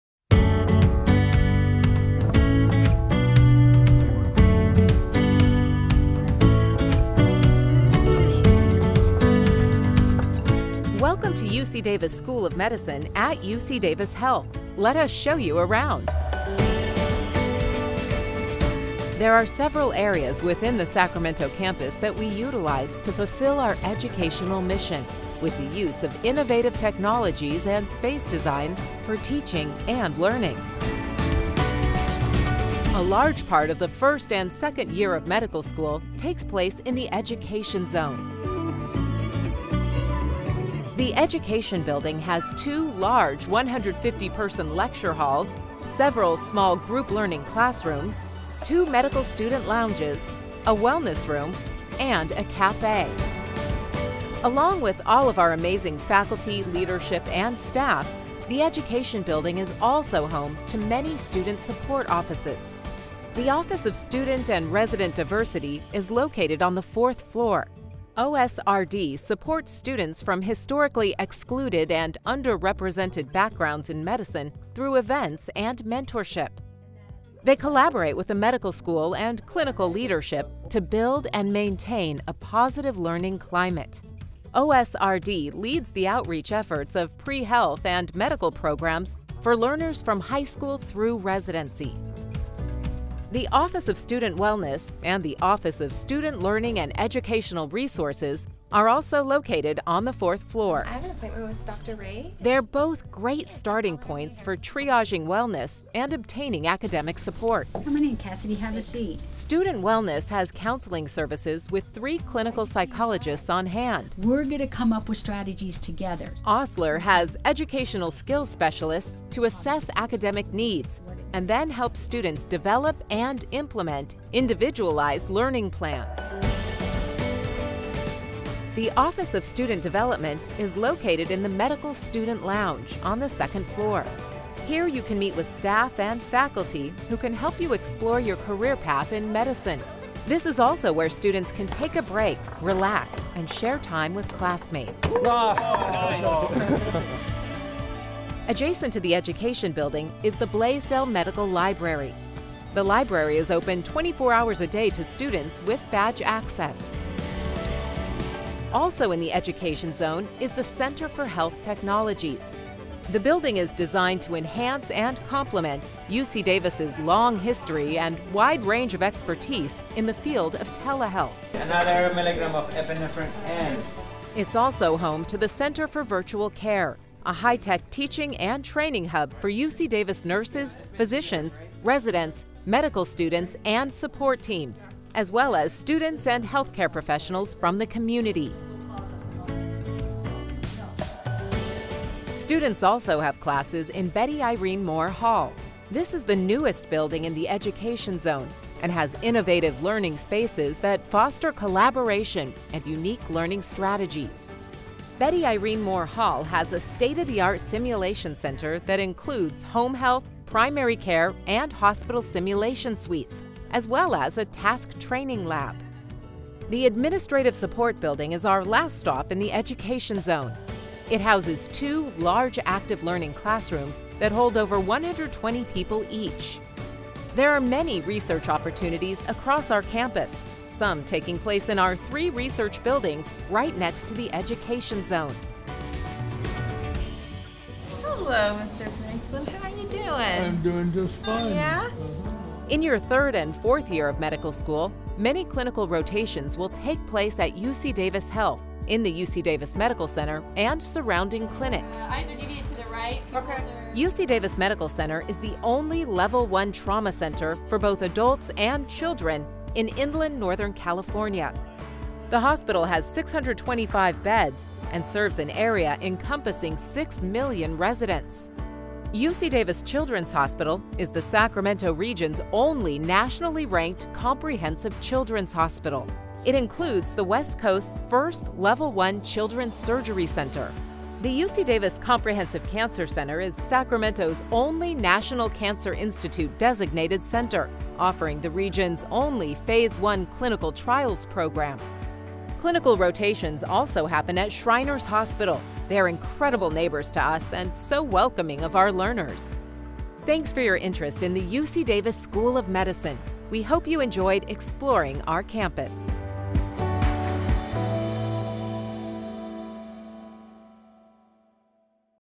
UC Davis School of Medicine Virtual Tour
Welcome to UC Davis School of Medicine at UC Davis Health. This virtual tour will take you around the Sacramento campus' world-class learning facilities and into the UC Davis Medical Center, the only level I trauma center for both adults and children in inland Northern California.

0:00 Welcome to UC Davis School of Medicine
0:30 The Sacramento Campus
1:05 Office of Student and Resident Diversity
1:32 Office of Student Wellness and Office of Student Learning and Educational Resources
2:09 Office of Student Development
2:30 Blaisdell Medical Library
2:38 Center for Health and Technology
3:12 Betty Irene Moore Hall
3:37 Administrative Support Building and Research Opportunities
4:00 Clinical Rotations